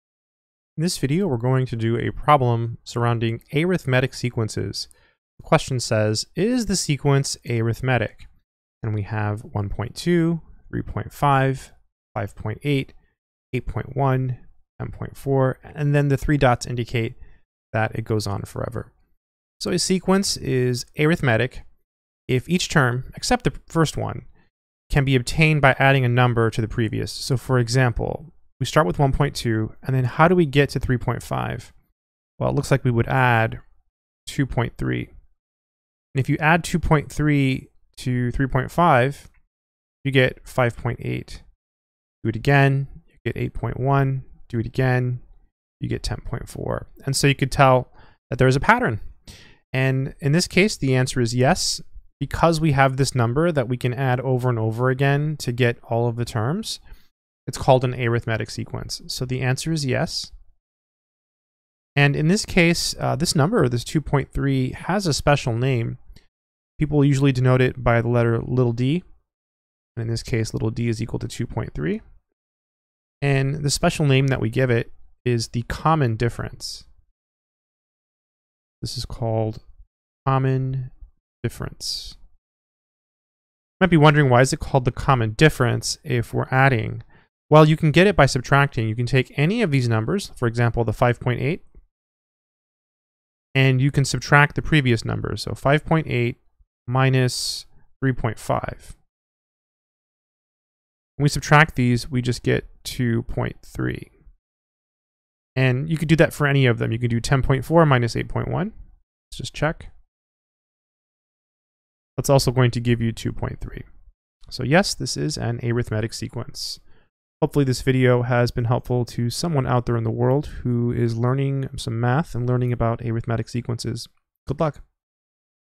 Arithmetic Sequence with Decimals
In this video we solve a math problem. We are given a sequence of decimals. We are asked to determine if it as arithmetic sequence. I hope this helps someone studying sequences.